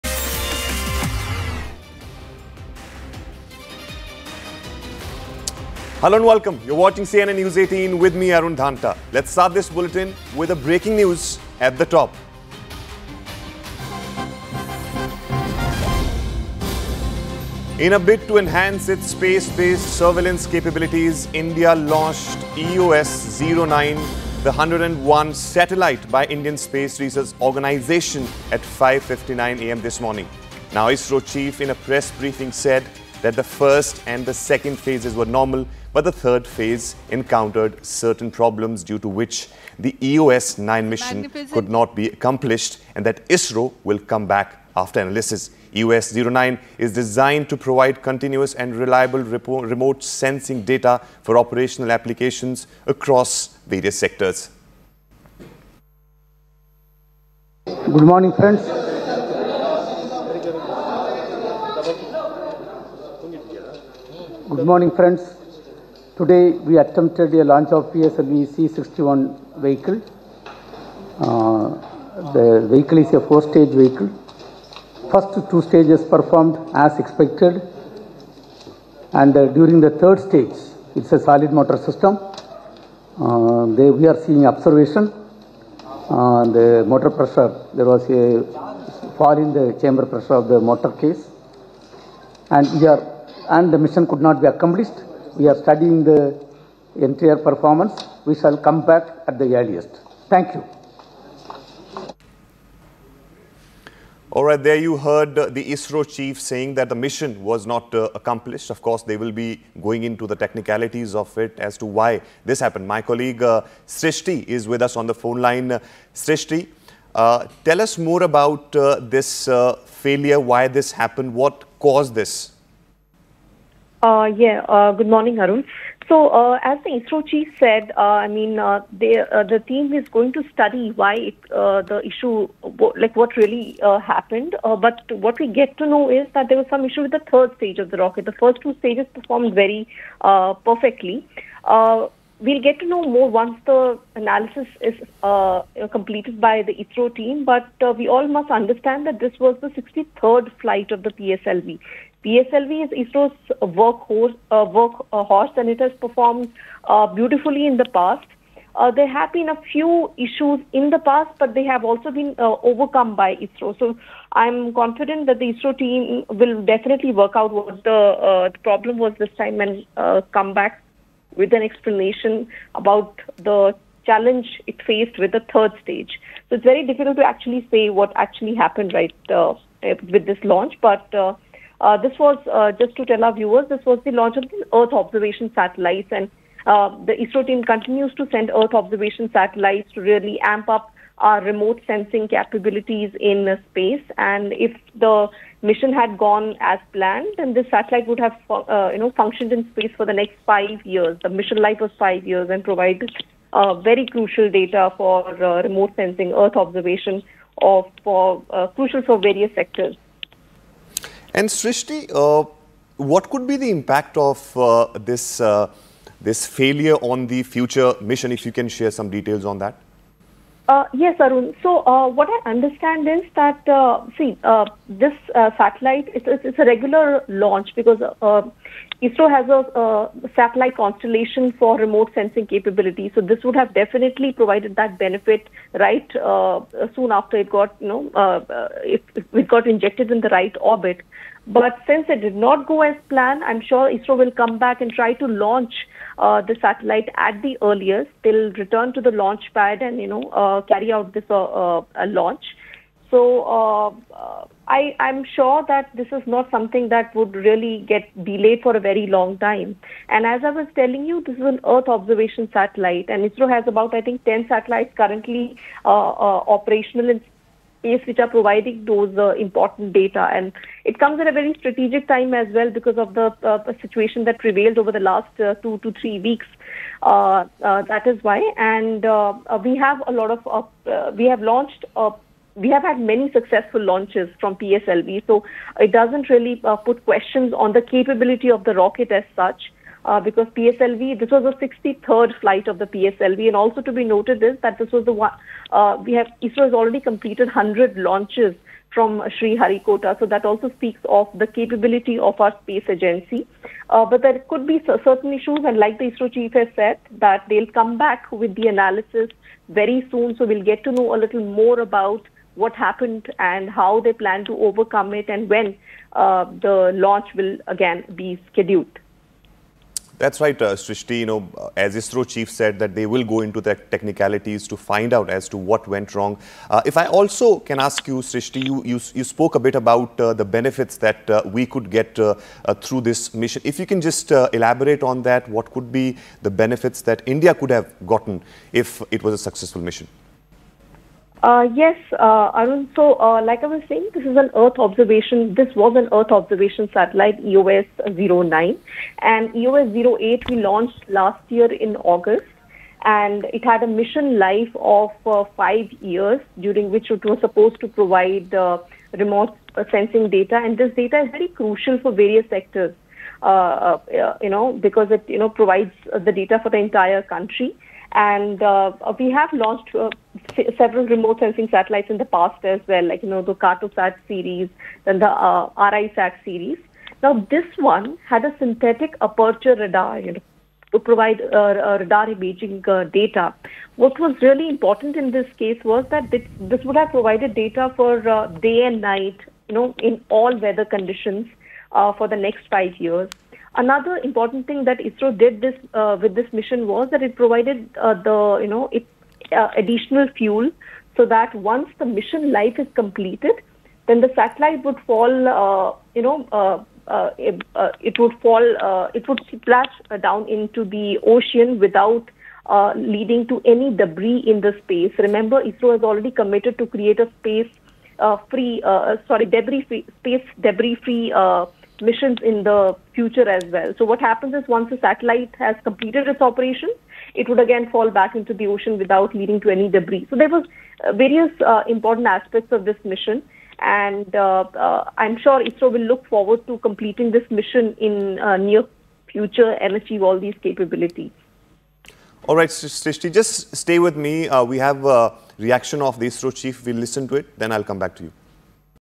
ISRO's PSLV-C61 Mission Carrying EOS-09 Failed Due To An Issue In The Third Stage | News18
ISRO's PSLV-C61 mission carrying EOS-09 failed due to an issue in the third stage. The launch was normal up to the second stage. "Mission could not be accomplished," confirmed ISRO chief. Investigation is underway.

n18oc_breaking-news